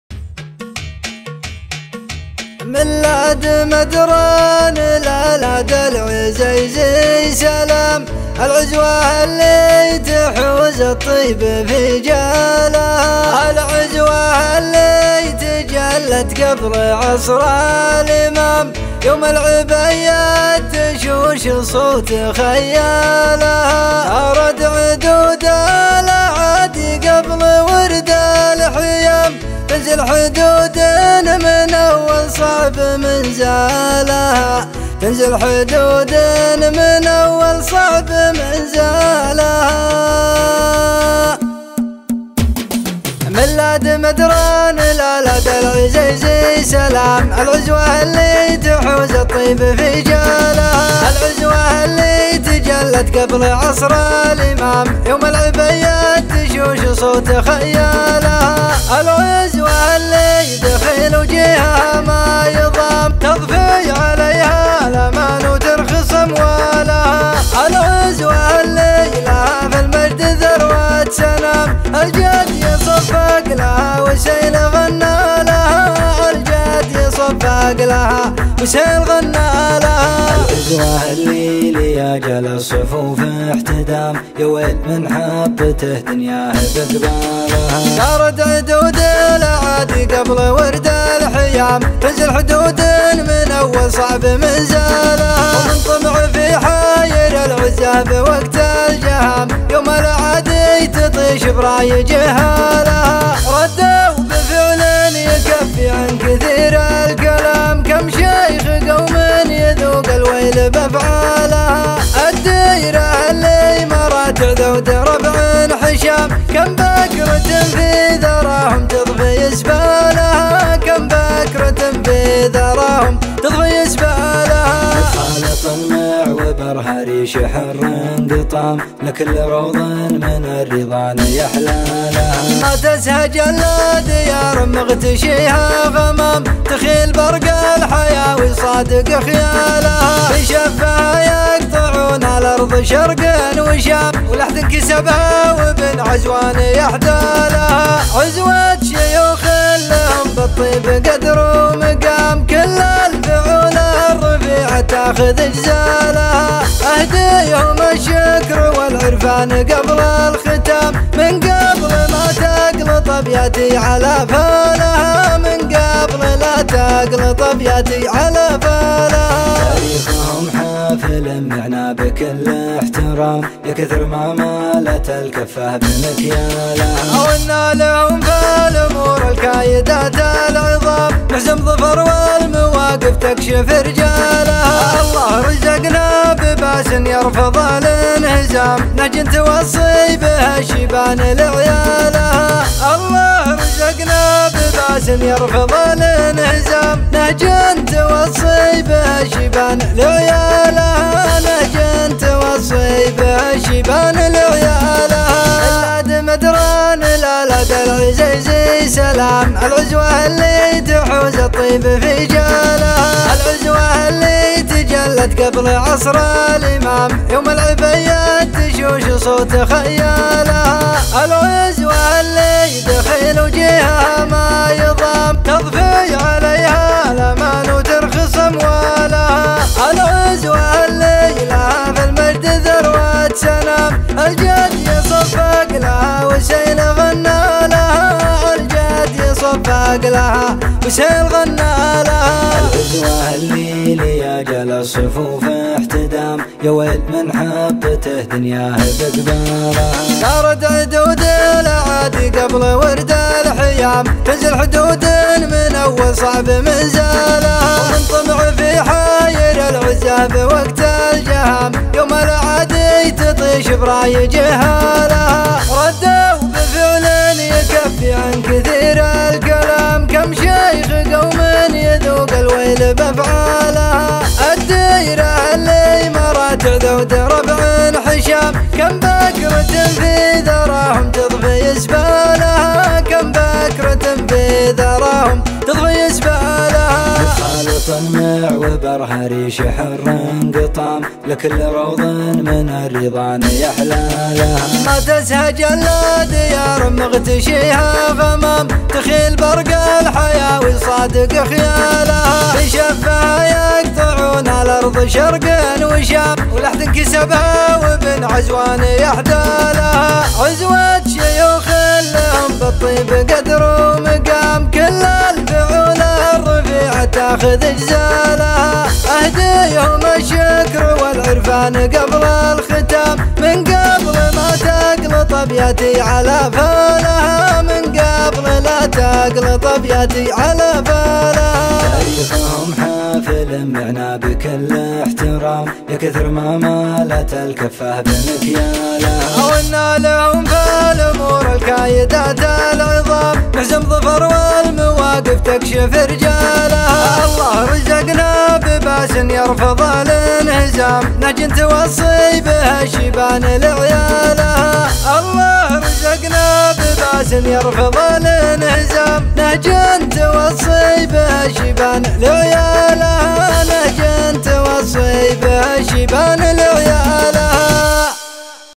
قصيده مهداه من الشاعر عايض عبدالله ابوشيبه لقبيلة العزه من سبيع ( الاد عزوان)
شعراء المملكه / شعراء الخليج / شعراء العرب /شعراء اسبيع استديوهات انتاج وتوزيع اعمال فنية واعلامية صوتية ومرئية